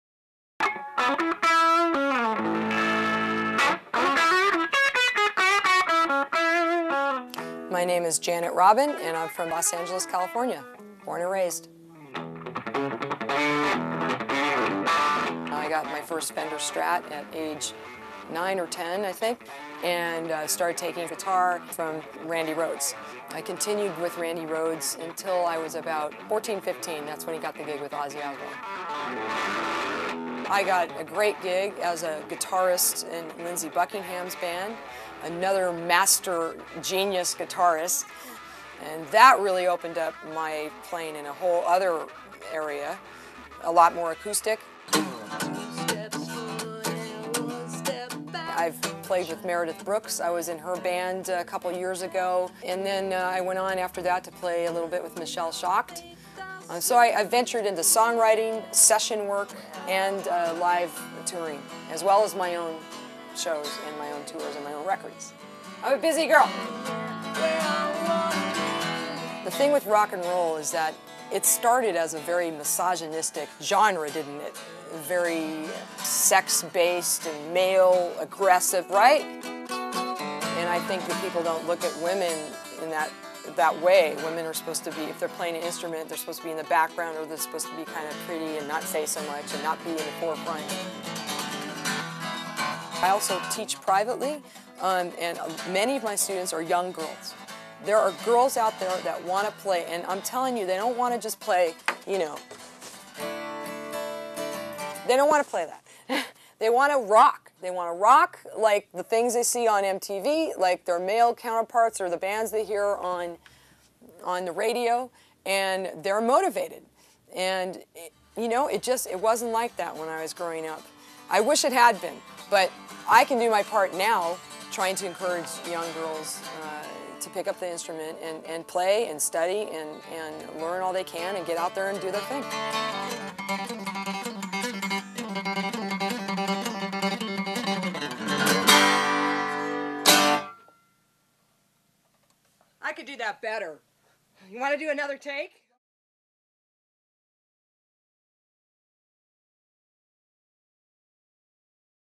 Janet Robin Interview on Girls with Guitars
Janet talks about her career in the music biz as a guitarist and singer/songwriter.